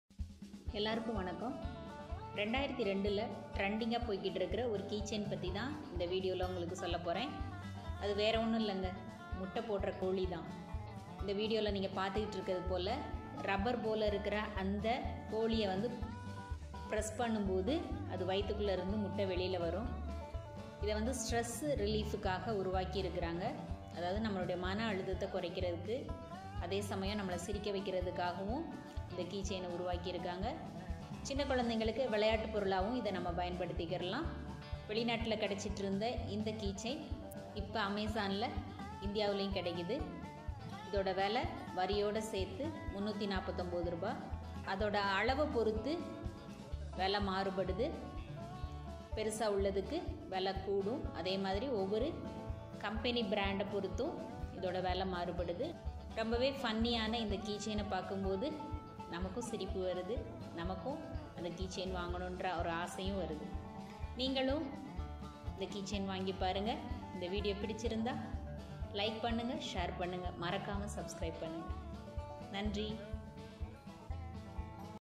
Weird Amazon Products | Keychain Gifts | Stress relief Keychain | Trending Keychain | Gift Ideas
@Estate Cooking
Welcome to Estate Cooking channel !!!
Egg Laying Chicken Keychain! Trending Keychain Gifts!
There is an egg in the chicken ,when you squeeze the chicken and then an egg pops out.
Hook and grab them, push and pull them, and pop them out, with a clear click sound, it sounds like a bubble wrap, which relieves your stress and anxiety
Most of these irritating hand-held toys are small and light, and can be placed in bags or pockets; Using mini decompression toys can enhance the coordination and creativity of hands and brains; They can also be used as travel toys to share with family and friends, so that you can play happily together.
Our fidgety toys are made of silicone and ABS, which are nontoxic and pollution-free. Suitable for children over 3 years old, with smooth edge, no burr and no BPA.
Suitable for people of all ages. It helps relieve stress and maintain a stable mood. It's also a great gift for Valentine's Day, Halloween, New Year's Day and Christmas, girlfriend.